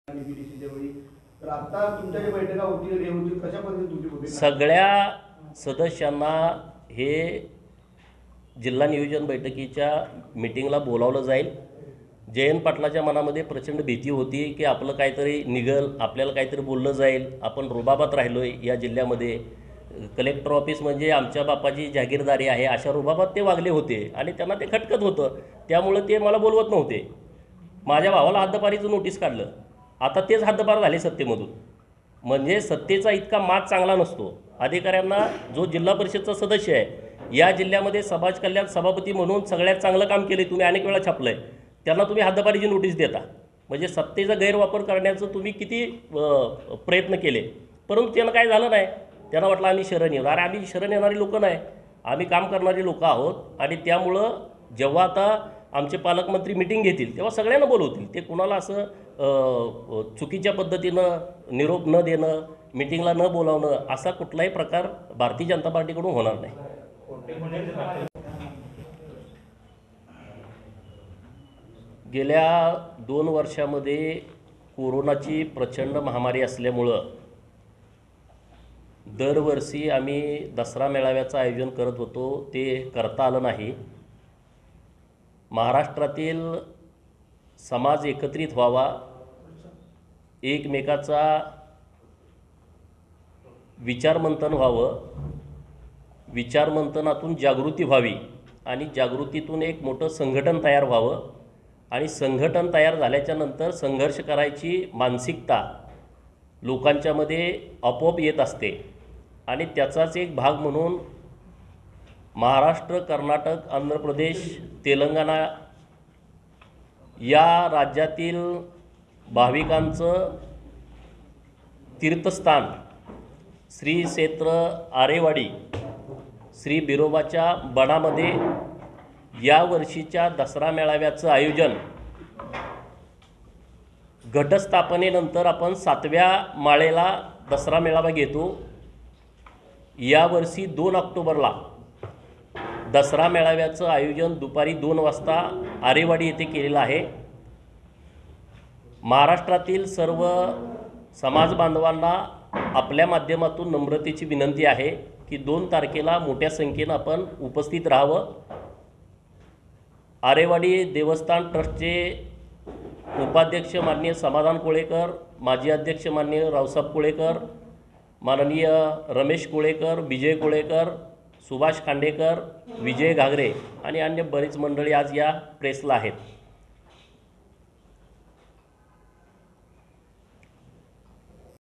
SANGLI | जिल्हाधिकारी कार्यालय आपल्या बापाची जहागिरीदारी असल्याप्रमाणे जयंत पाटील वागले - पडळकर
पालकमंत्री असताना, सांगली जिल्हाधिकारी कार्यालय आपल्या बापाची जहागिरीदारी असल्या प्रमाणे जयंत पाटील वागले.  माझ्या भावावर हद्दपारीची नोटीस काढणारे जयंत पाटील हेच आता सत्तेतून हद्दपार झाले आहे.  सत्तेचा इतका माज चांगला नसतो, अशी टीका भाजपा आमदार गोपीचंद पडळकर यांनी केली. आम्ही काम करणारी लोकं आहोत. आमचे पालकमंत्री सगळ्यांना बोलावतील. निरोप न देणं आणि न बोलावणं अशा कोणताही प्रकार भाजपाकडून होणार नाही.  असे गोपीचंद पडळकर यांनी निक्षून सांगितले. सांगलीमध्ये आमदार गोपीचंद पडळकर हे प्रसारमाध्यमांशी बोलत होते.

        सांगली जिल्ह्यातील आरेवाडी येथे दोन ऑक्टोबर रोजी "दसरा मेळाव्याच आयोजन करण्यात आले आहे. या मेळाव्याला महाराष्ट्र कर्नाटक आंध्र सहित अनेक राज्यातील धनगर बांधव मोठ्या संख्येने उपस्थित राहतील, अशी माहिती पडळकर यांनी यावेळी दिली. धनगर समाजाच्या विविध प्रश्न, अडचणीवर मेळाव्यात चर्चा होणार असल्याचेही त्यांनी यावेळी सांगितलं.

BYTE - आमदार गोपीचंद पडळकर